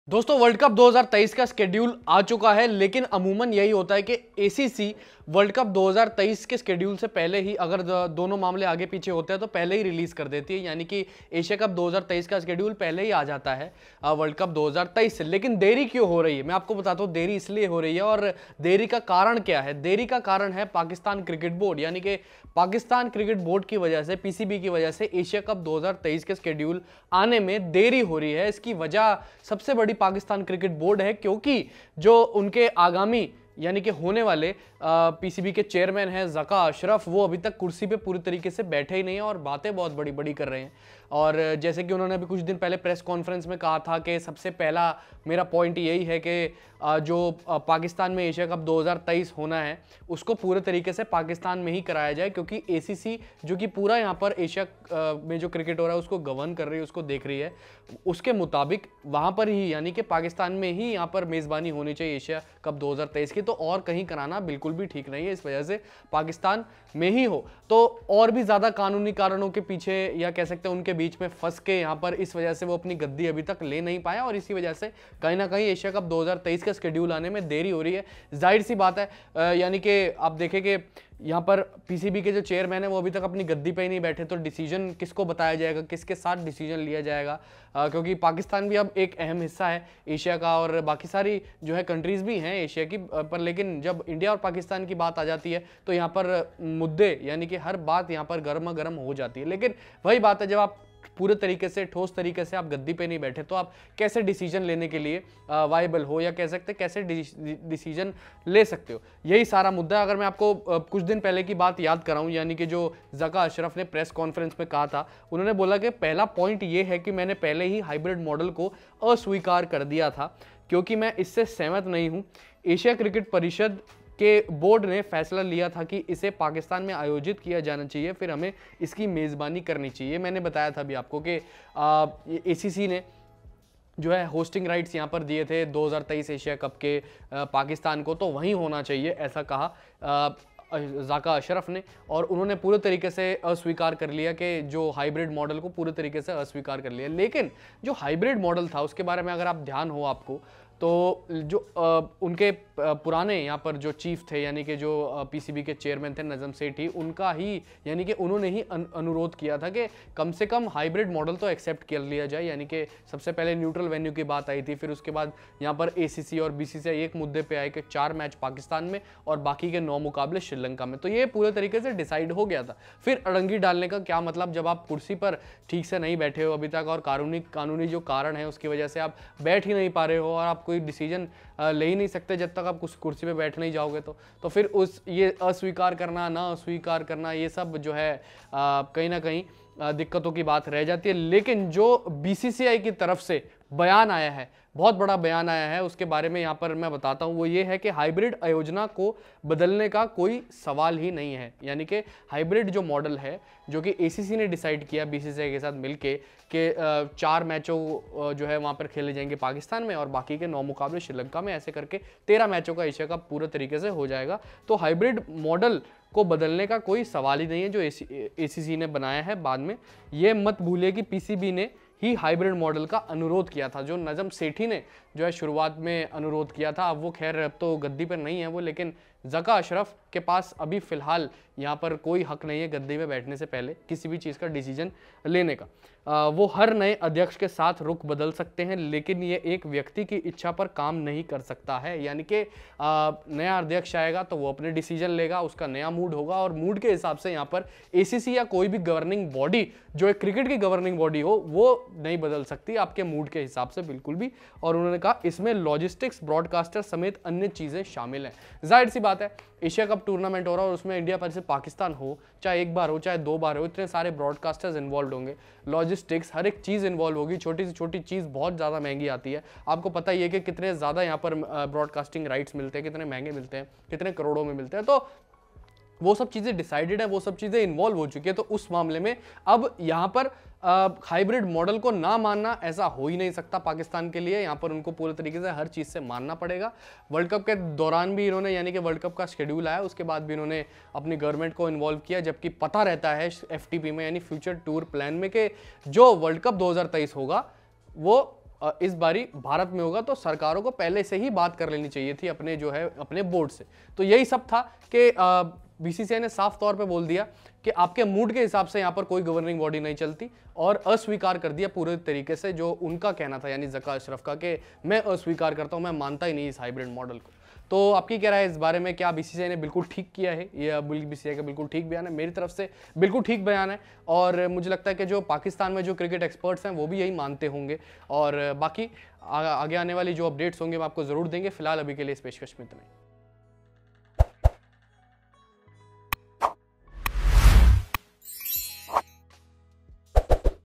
PCB की वजह से हो रही है Asia Cup 2023 Schedule आने में देरी | Zaka Ashraf | IND Vs PAK | BCCI
Anchor: Ritik Kapoor
Editor: Raj Pal